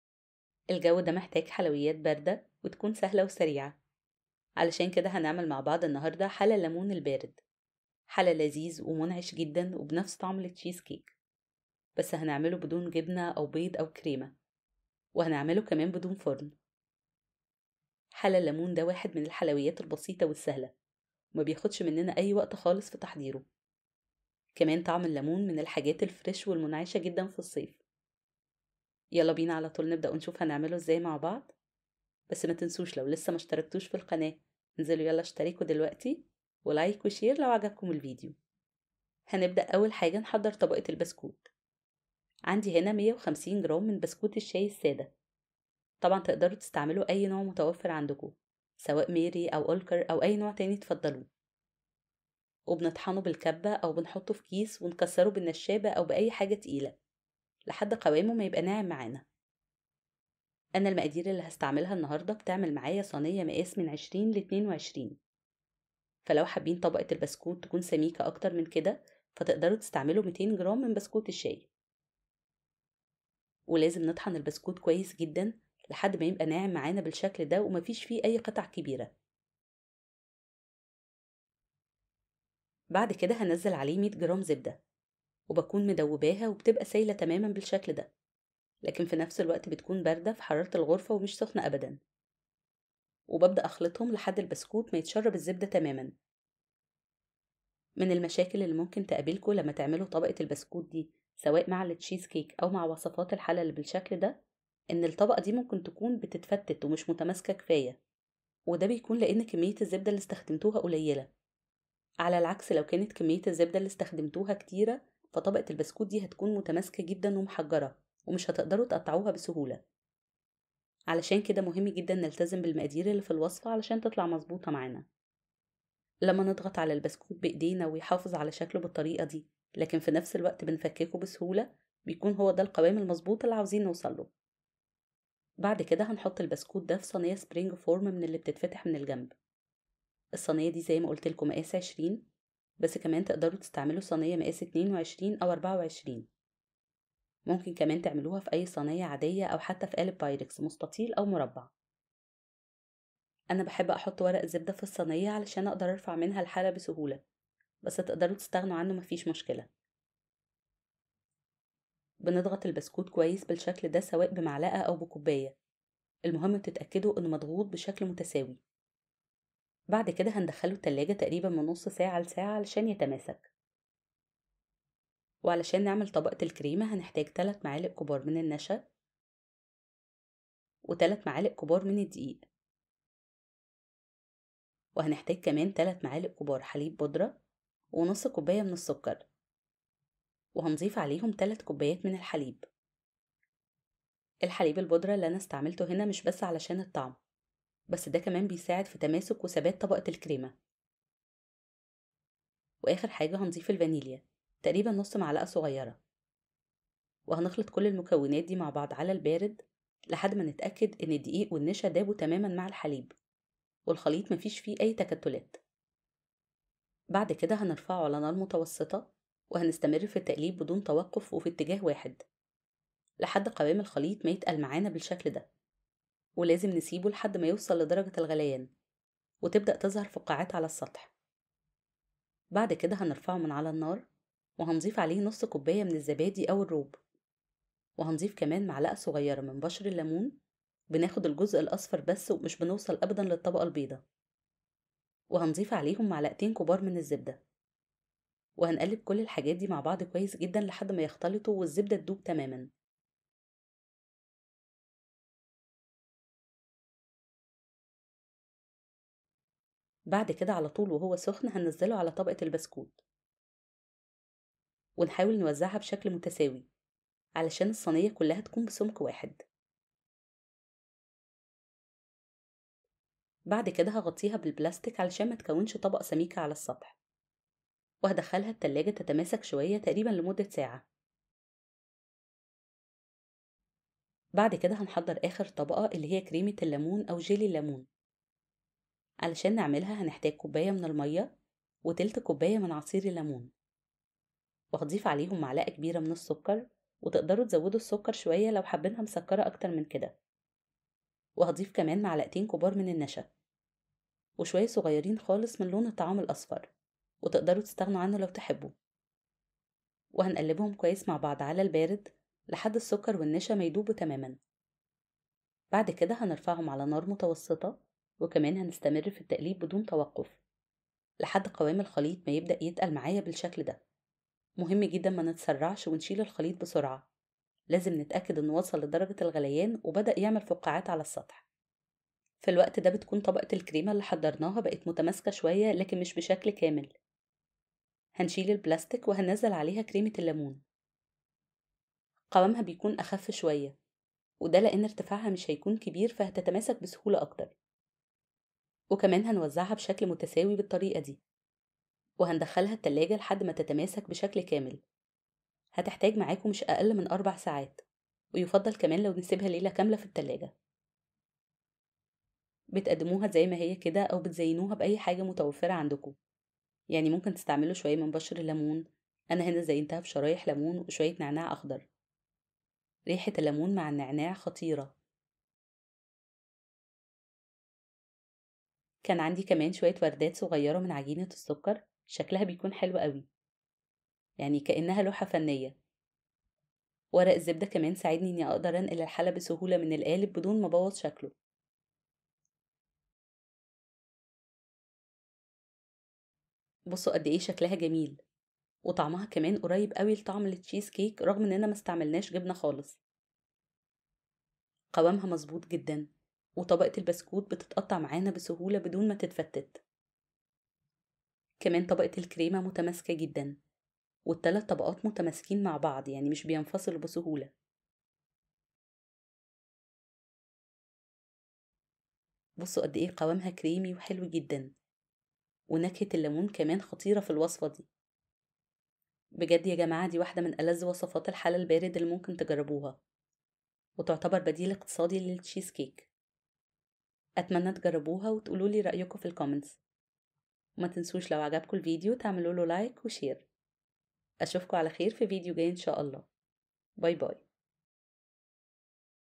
حلا الليمون البارد و المنعش طعمه تشيز كيك بالظبط بدون جبنه، كريمه ولا فرن ويتحضر ف دقايق
المقادير :
لعمل طبقة البسكويت
150 جرام بسكويت سادة
100 جرام زبدة

لعمل طبقة الكريمة
3 ملعقة كبيرة دقيق
3 ملعقة كبيرة نشا
3 ملعقة حليب مجفف
1/2 كوب سكر
3 كوب  حليب
نصف كوب زبادي
ملعقة صغيرة بشر ليمون
فانيلا
لعمل طبقة كريمة الليمون
كوب ماء
1/3 كوب عصير ليمون
ملعقة كبيرة سكر ( يمكن اضافة المزيد حسب الرغبة )
2 ملعقة كبيرة نشا
لون طعام اصفر

إذا كنتم ترغبون في متابعة المزيد من وصفاتنا المبتكرة

 شاركوا الفيديو مع أصدقائكم وعائلتكم لتستمتعوا بتجربة الطهي معًا! 🤗👨‍🍳👩‍🍳